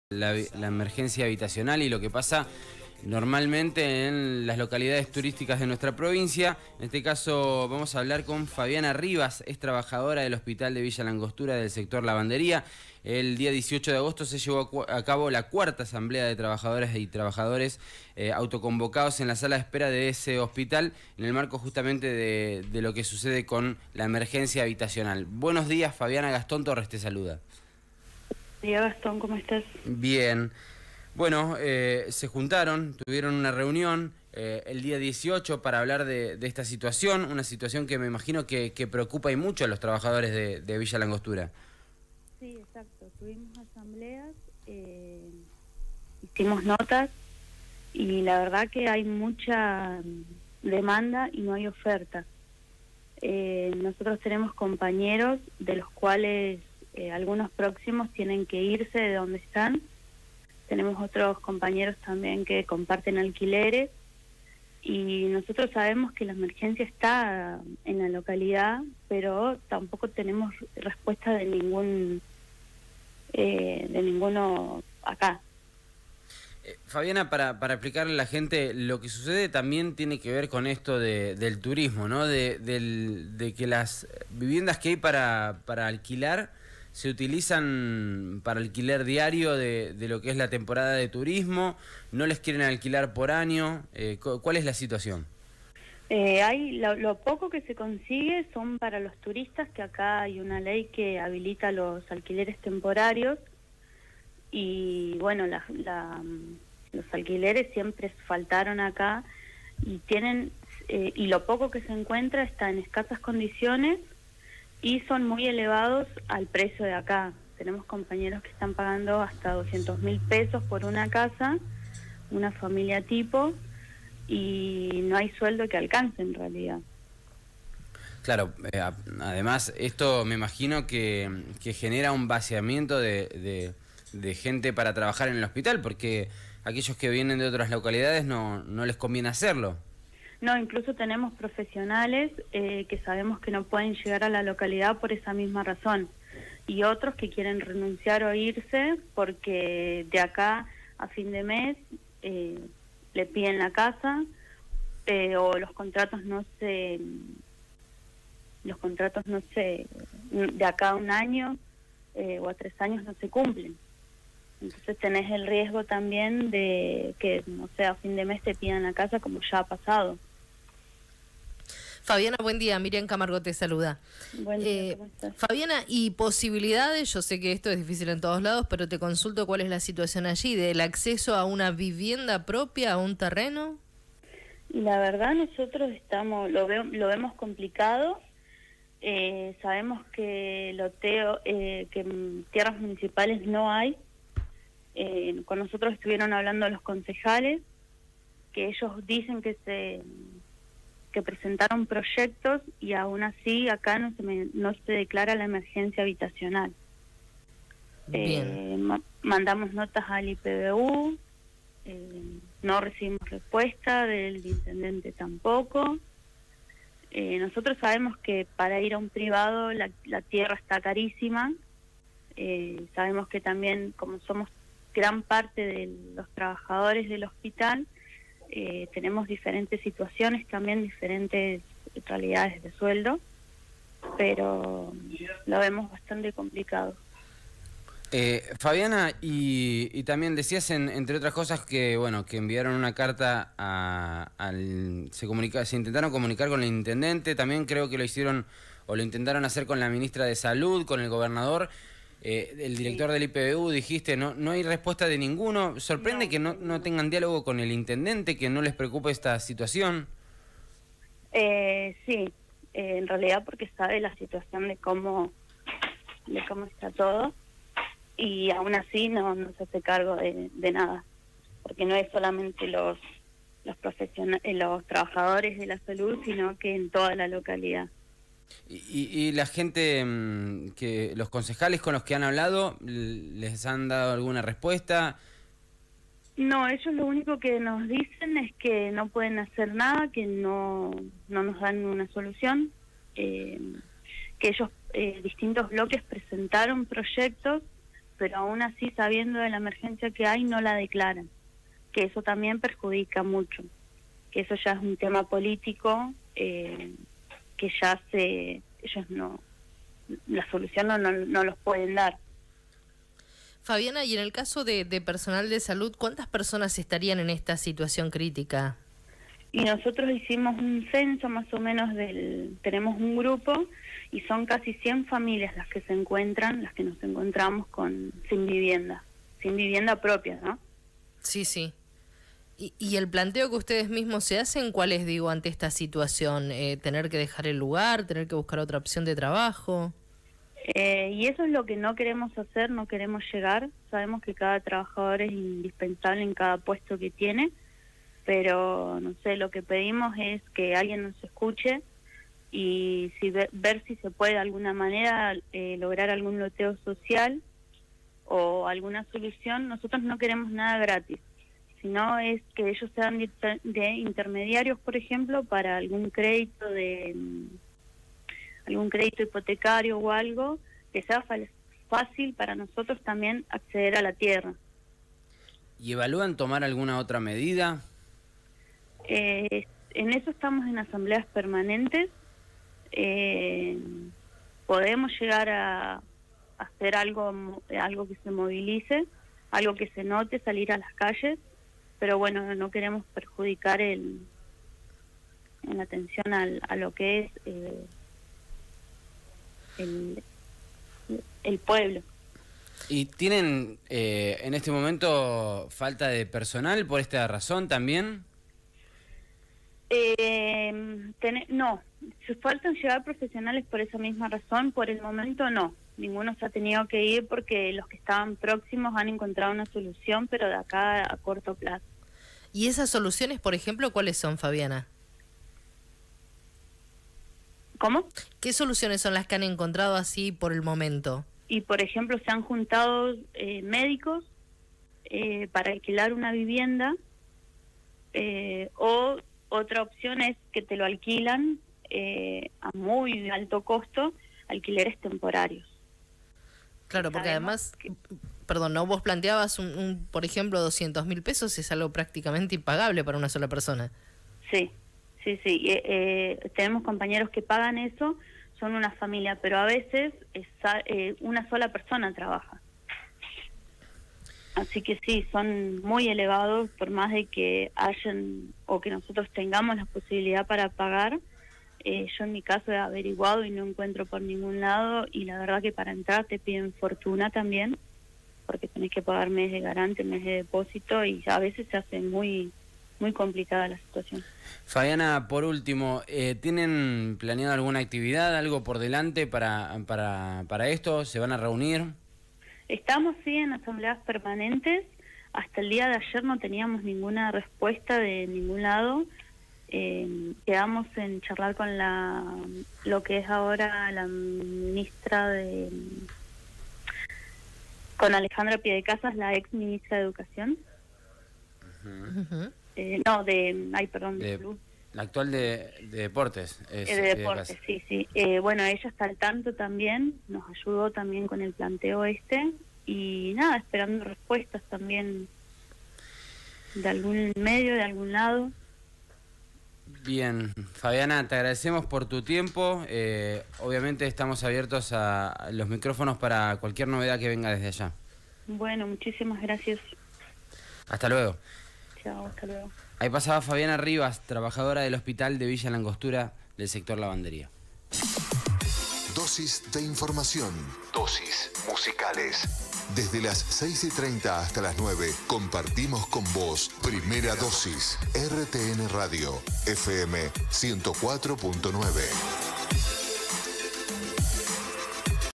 Complicaciones habitacionales para profesionales de la salud en Villa la Angostura
NOTA TELEFÓNICA A FABIANA RIVAS, TRABAJADORA HOSPITAL VILLA LA ANGOSTURA – SECTOR LAVANDERÍA

TEMA: El día 18 de agosto se llevó a cabo la cuarta asamblea de trabajadores y trabajadoras autoconvocados en la sala de espera del hospital de Villa la Angostura, en el marco de la emergencia habitacional que atraviesa la localidad. En la misma se conversó sobre la dificultad para acceder a la vivienda propia, los costos altísimos de los alquileres y la falta de políticas públicas para combatir esta problemática. Casi 100 familias son las que contabilizaron, qué se encuentran en emergencia habitacional, algunos próximos a dejar sus alquileres, otros compartiendo vivienda y muchos otros qué se encuentran en la búsqueda de un alquiler permanente. Está realidad que afecta a toda la comunidad Angostura, no deja de lado a los trabajadores de salud, quienes se han comunicado con distintos concejales y conversado en el patio del hospital, que manifestaron no poder dar ninguna solución al respecto. Por otro lado, hemos enviado nota a la ministra de salud Andrea Peve, al gobernador Omar Gutiérrez, y al director del IPVU, poniéndolos al tanto de la situación de los trabajadores profesionales y no profesionales de salud. También han realizado reiterados pedidos para reunirse con el intendente Fabio Stefani quién aún no ha dado respuesta alguna, con el fin de dialogar y dar solución a las necesidades de cada uno de los miembros del hospital.